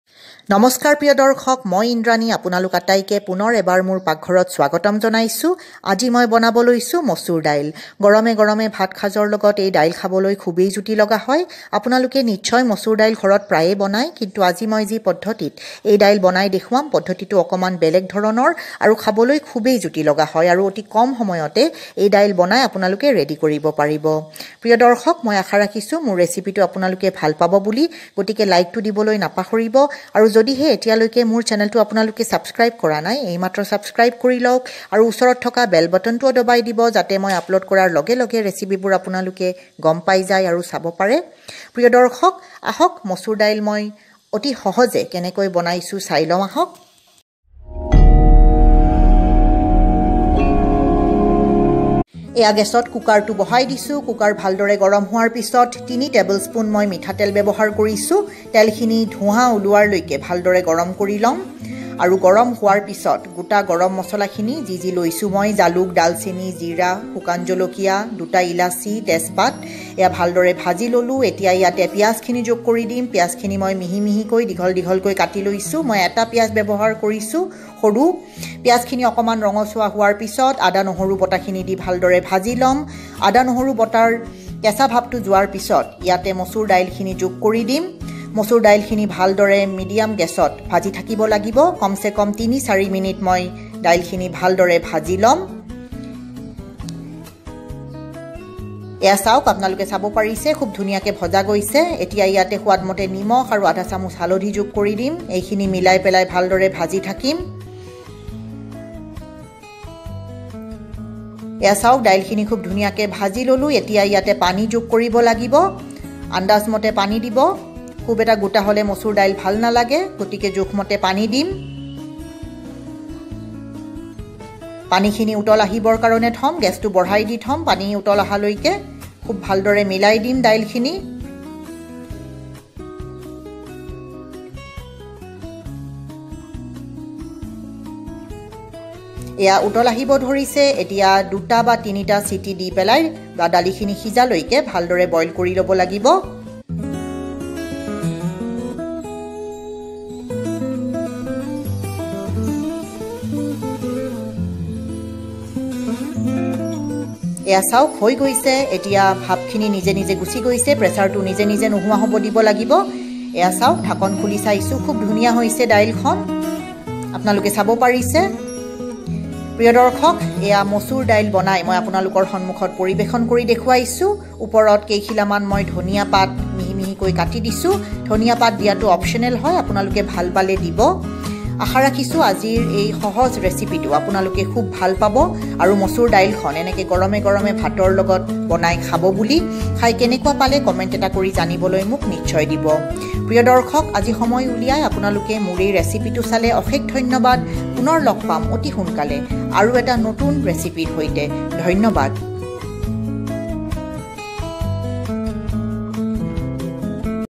এনেকৈ মচুৰ দালি বনাই খাইছেনে | Masoor Dali recipe in Assamese | Assamese Dal Recipe
Very tasty and healthy Dal Recipe in Asamese language.  You can prepare this simple recipe within few minutes.
 Enjoy it with steamed rice and I am sure you will never forget this experience.